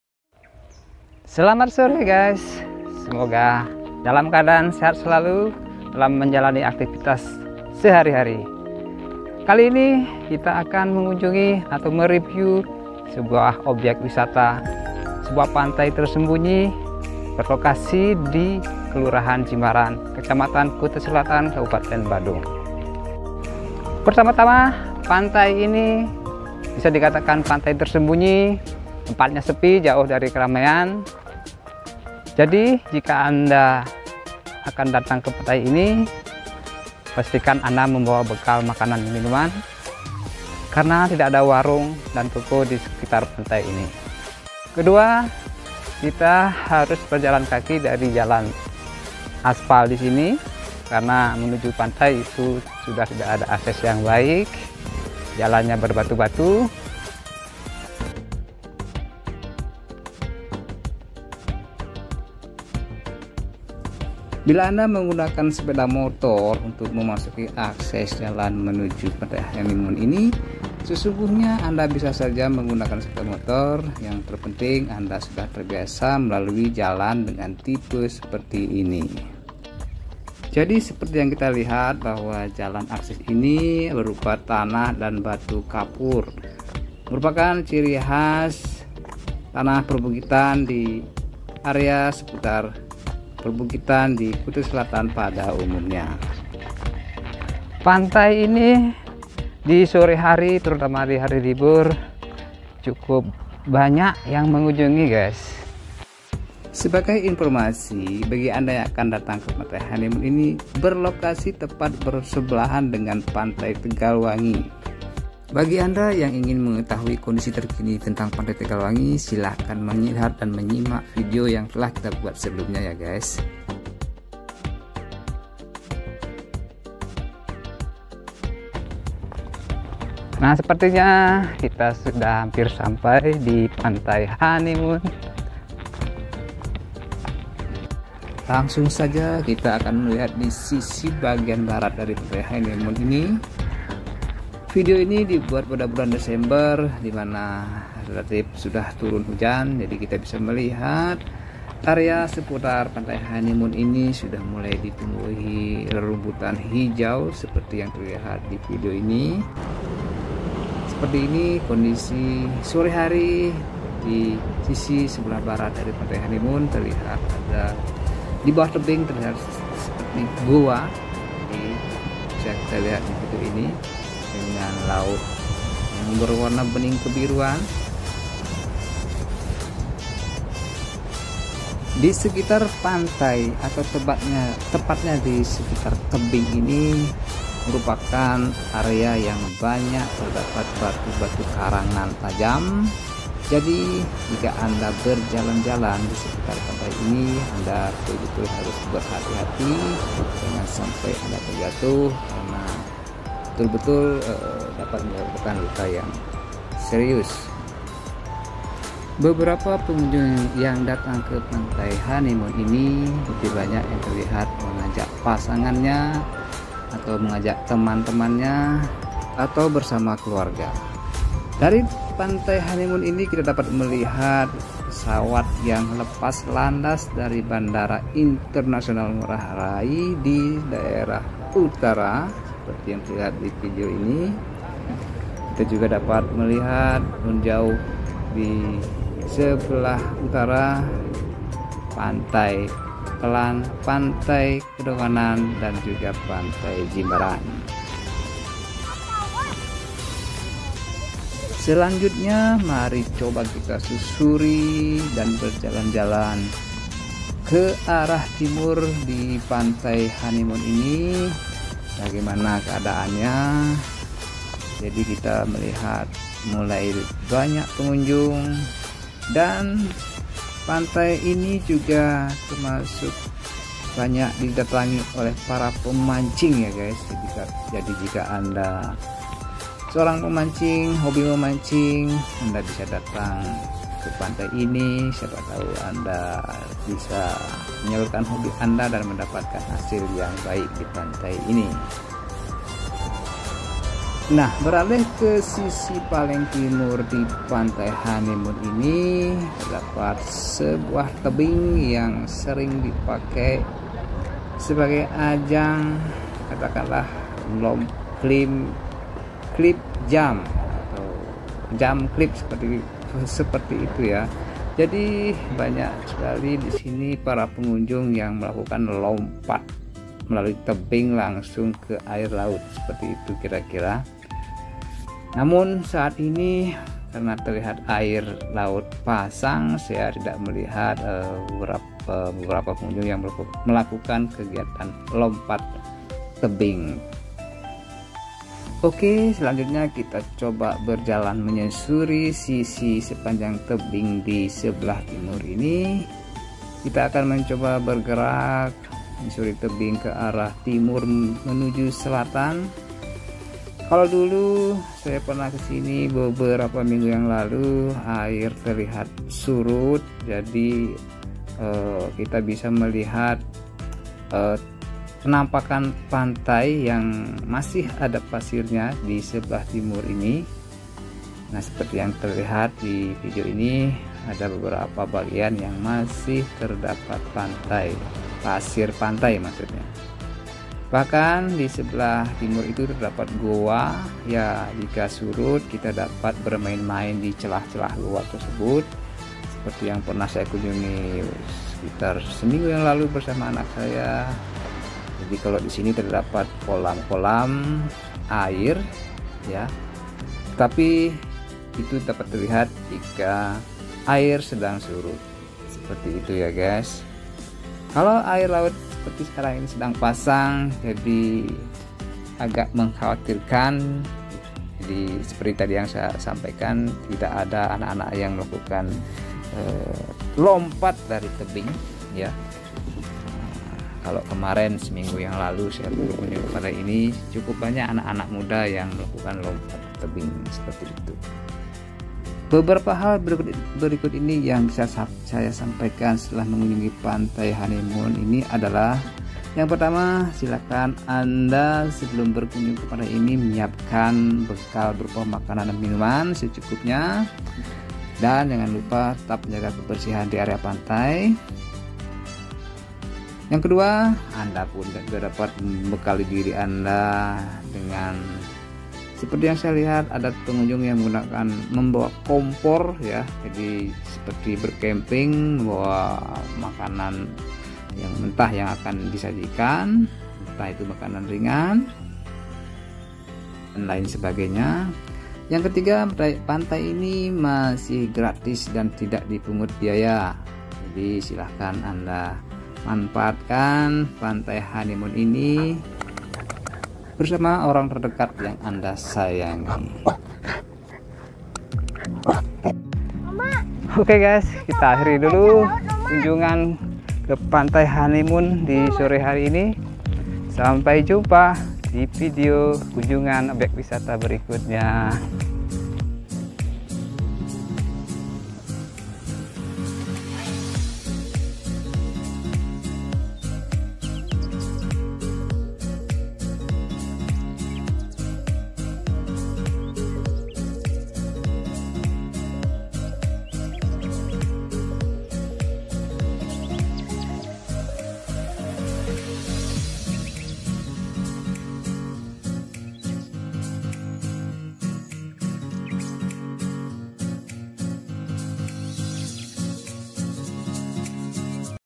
Jelajah Pantai Honeymoon di Jimbaran, Kuta Selatan (Re-Upload)
Pantai Honeymoon berlokasi di kel. Jimbaran, kec. Kuta Selatan merupakan sebuah pantai yang berdampingan dengan pantai tegal wangi dengan panorama tebing, dan sedikit area berpasir di sisi timur selatan, salah satu pantai yang bisa anda kunjungi saat berlibur.